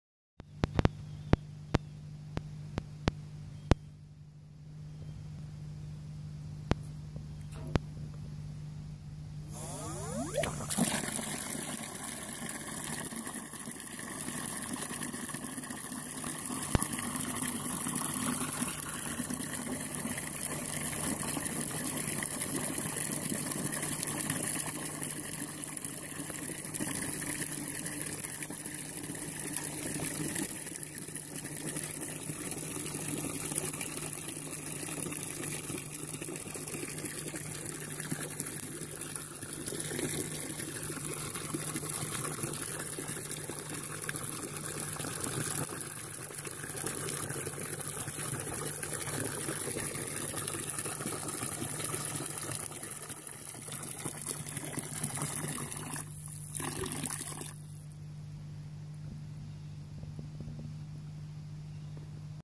Beautiful sound or noise of water coming up the pipe. Filling a bucket.
Listen to the sound of water filling up a bucket.